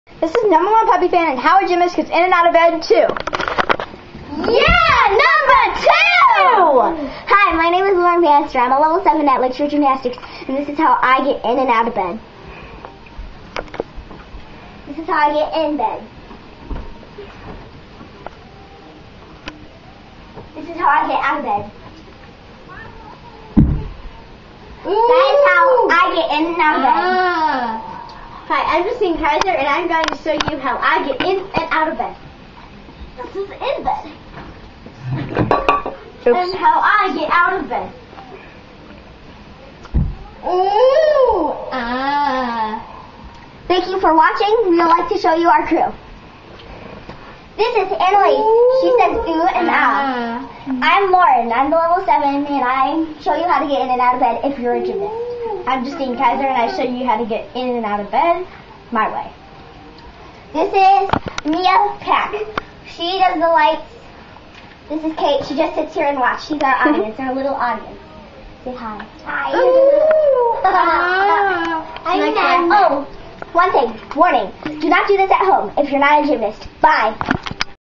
How a Gymnast Gets in and out of Bed 2!!!
Even better than the first! Starring my sister Justine, her friends Analice, Mia, Lauren, and Lauren's little sister Kate.